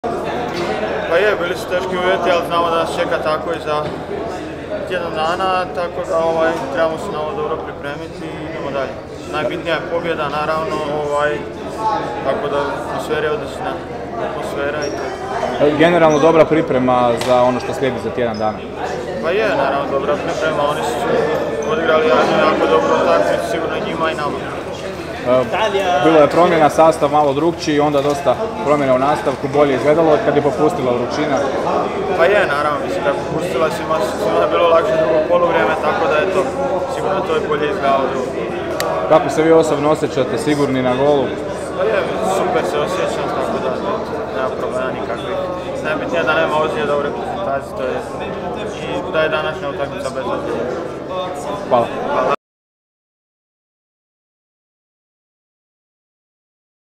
Izjava Dominika Livakovića nakon utakmice protiv Saudijske Arabije
Izjava Dominika Livakovića nakon prijateljske utakmice protiv Saudijske Arabije.